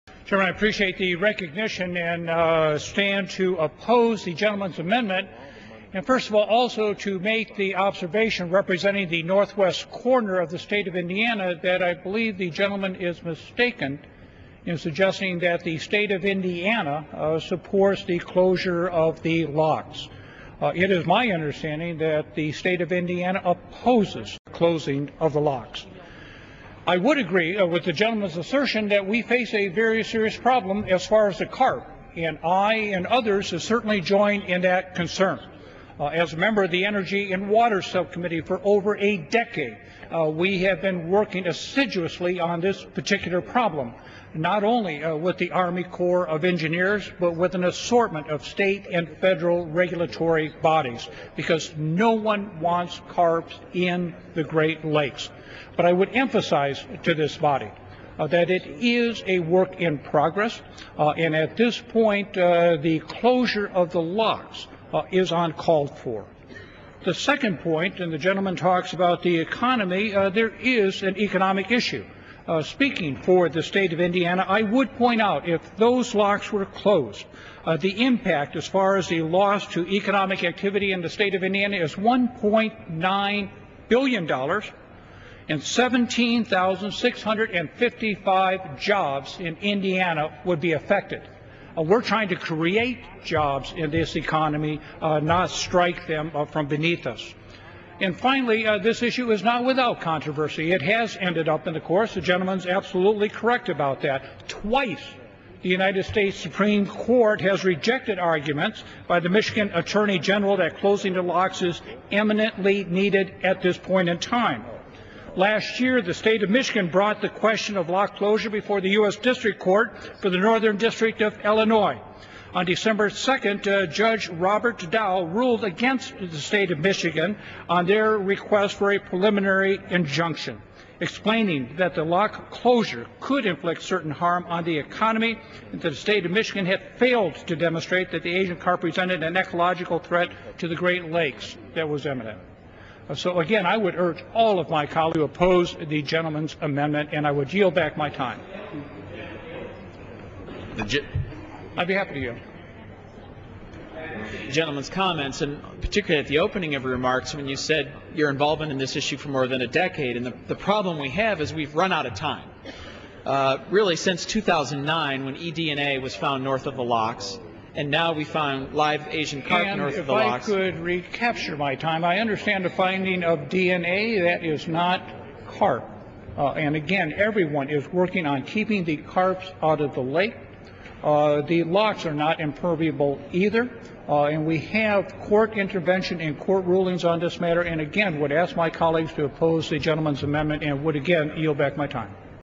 Visclosky Gives Floor Speech on Asian Carp
On February 17th, 2011, Pete spoke on the House floor to oppose an amendment (Amendment 516 to H.R. 1) from Rep. Dave Camp that would have prohibited funds to be used for the opening of the locks at the Thomas J. O'Brien Lock and Dam or the Chicago River Controlling Works. Read an excerpt of Pete's speech: "I would point out, if those locks were closed, as far as a loss to economic activity in the state of Indiana is $1.9 billion dollars and 17,655 jobs in Indiana would be affected. We're trying to create jobs in this economy, not strike them from beneath us."